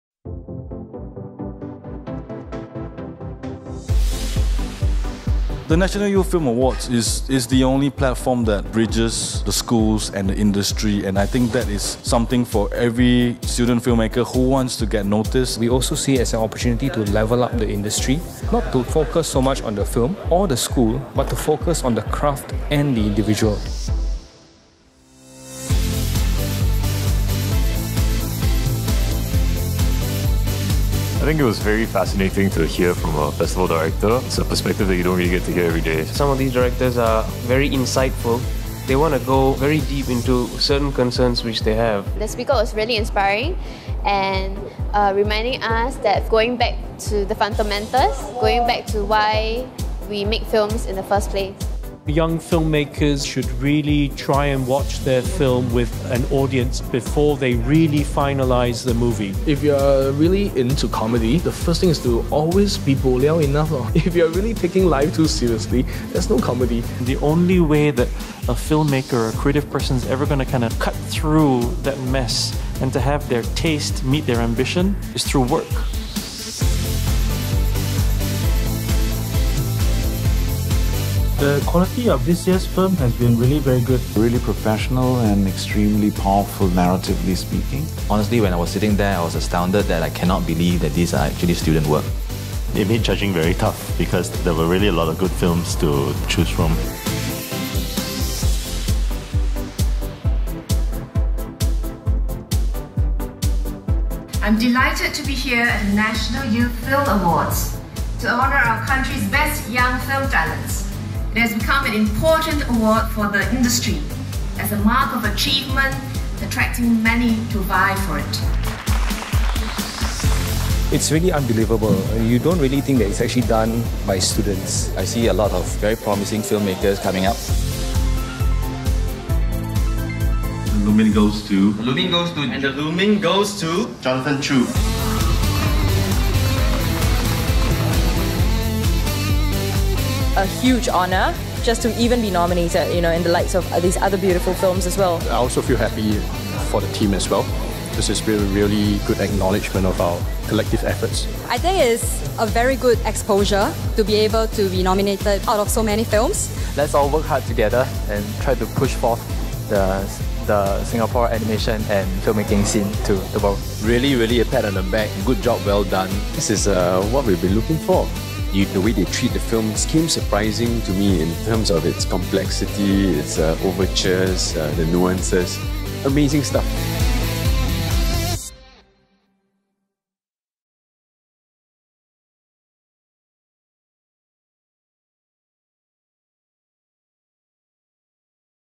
National Youth Film Awards 2016 Highlights
The spotlight shone on a new generation of film auteurs this afternoon, as a total of 20 awards were presented to 22 unique youth filmmakers from local Institutions of Higher Learning (IHLs) at the 2nd National Youth Film Awards (NYFA) Ceremony.

Organised by *SCAPE, the NYFA discovers and celebrates the best emerging talent for technical film crafts in Singapore, and aims to establish and raise a national benchmark for excellence in film. The annual ceremony was officiated by Guest-of-Honour Ms Grace Fu, Minister for Culture, Community and Youth.

Nanyang Technological University (NTU) bagged the Best Overall School Award for the second consecutive year, with 13 wins including the Best Visual Effects, Best Editing, Best Production Design, Best Direction, and the DBS Best Picture awards. The school had seen 55 nominees, the largest number of NYFA nominees this year from its Bachelor of Communication Studies, Bachelor of Fine Arts (BFA) in Digital Filmmaking, and BFA in Digital Animation courses.